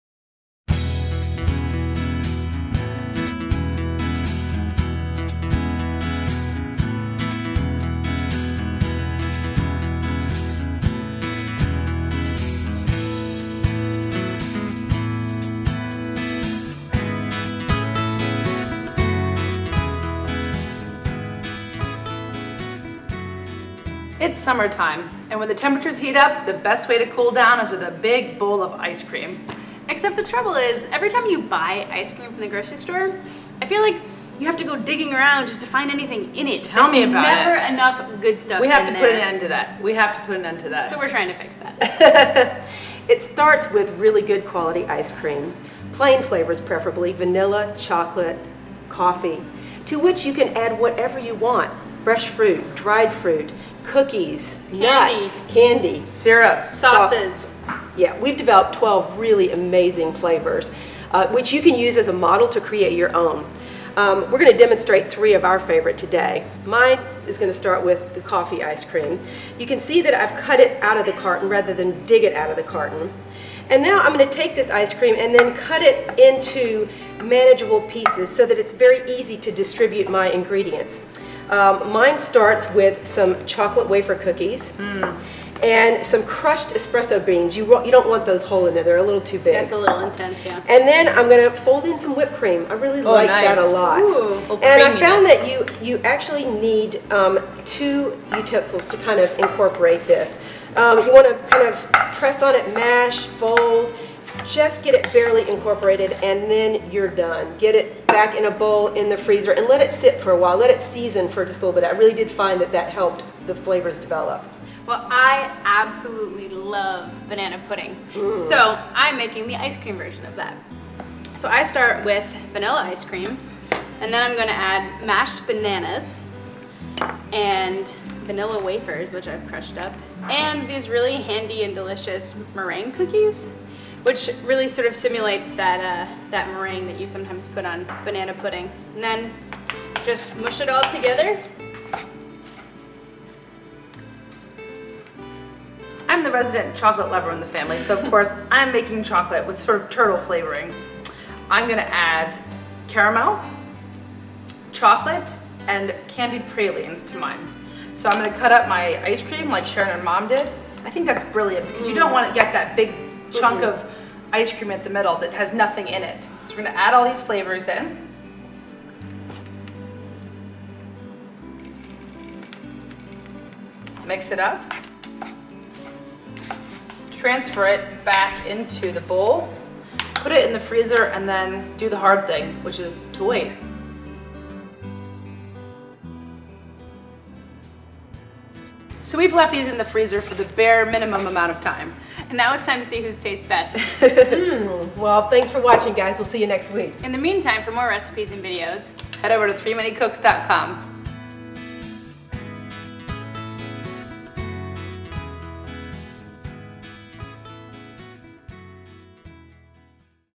Create Your Own Ice Cream
I love ice creams with fun names, but there's never enough good stuff in commercial brands. I want just enough ice cream to bind all the chunks. Plus, I like creating my own flavors. Here's the result of one of my trips to the grocery store. Follow what I've done or use them as a model to create your own.